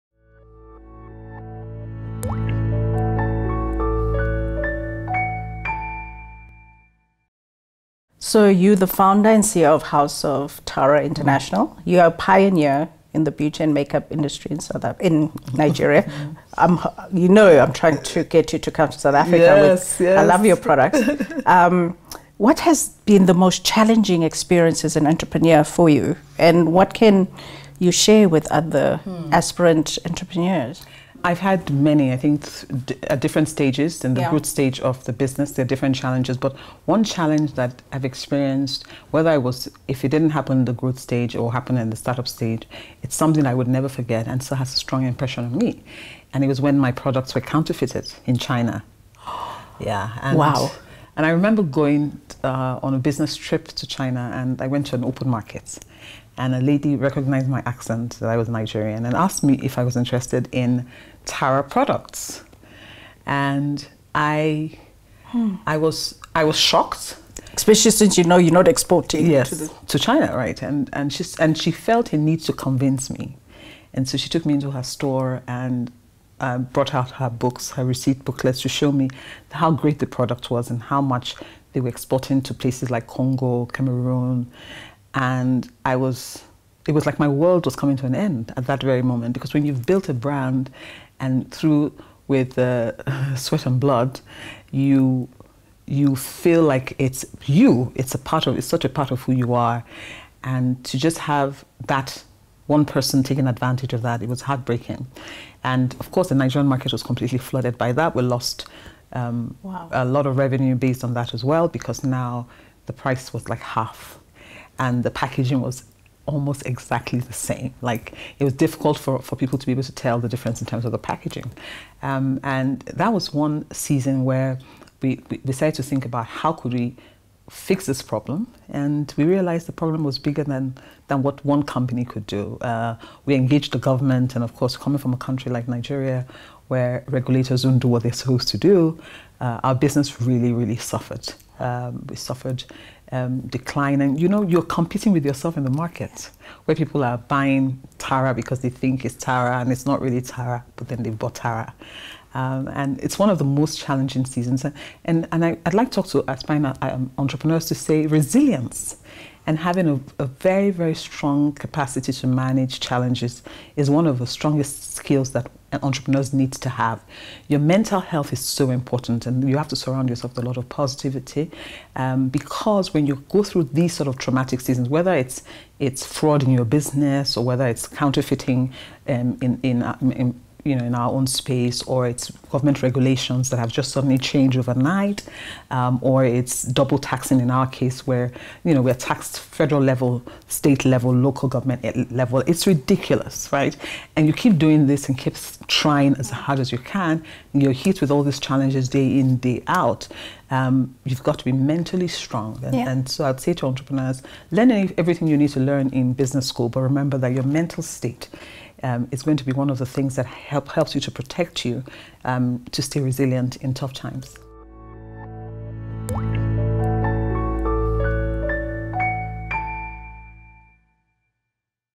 Tara Fela-Durotoye 17/2 WisdomPersonified with Dudu Msomi
Dudu Msomi discussing wisdom with Tara Fela-Durotoye. Insight1